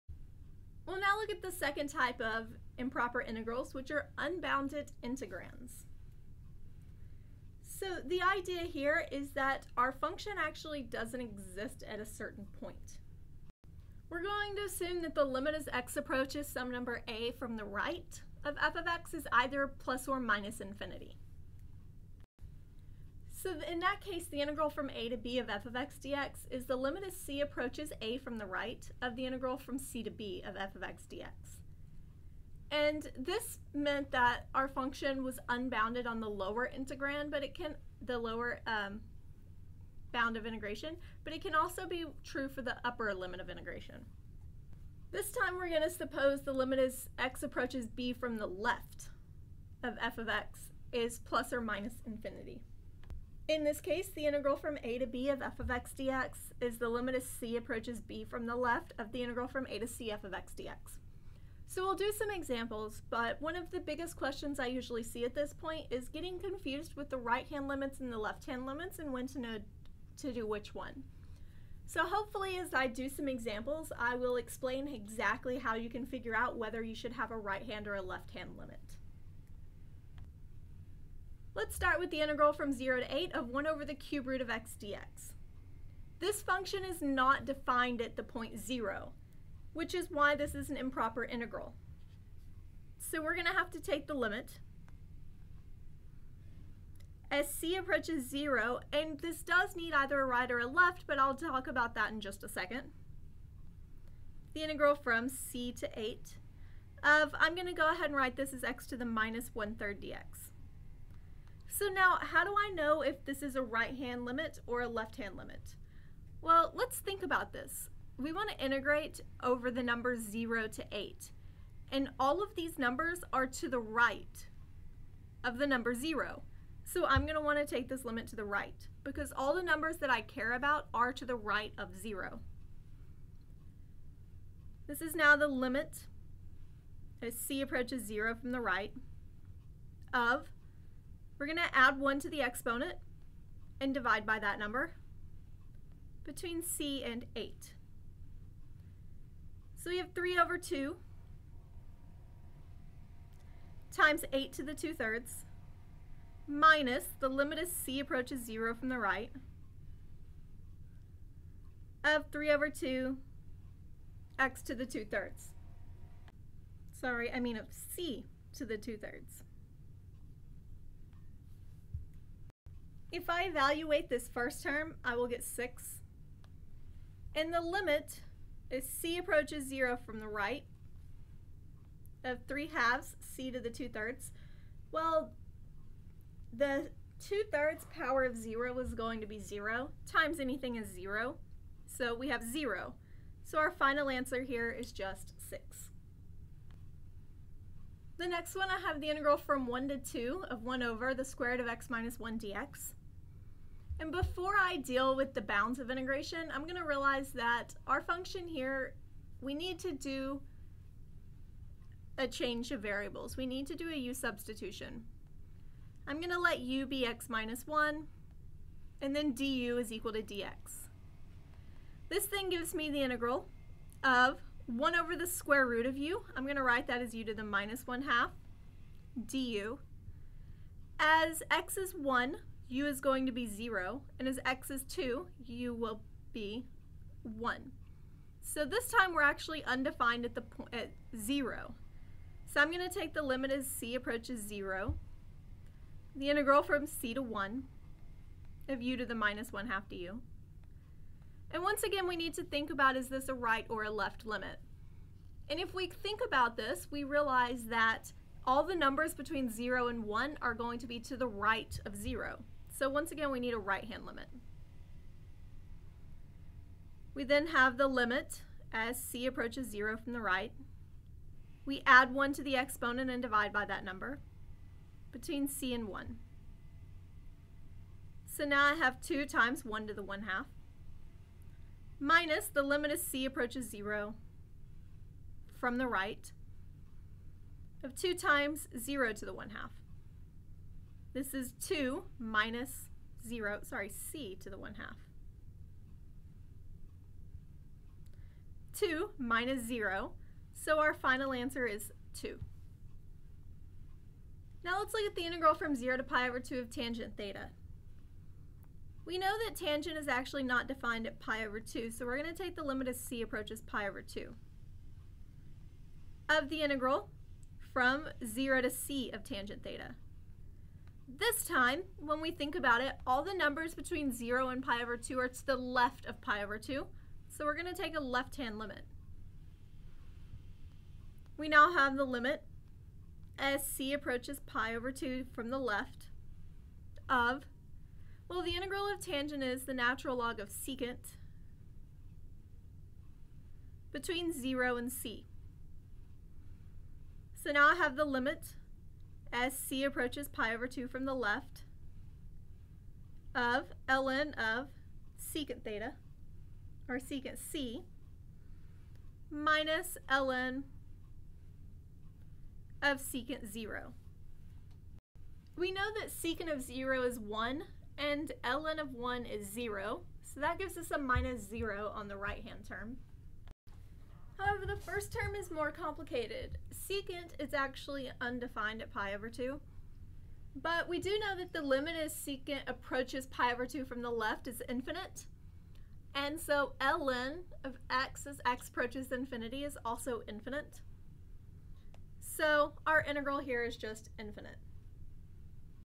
Improper Integrals with Unbounded Integrands
We define improper integrals over unbounded integrands and show how to solve these.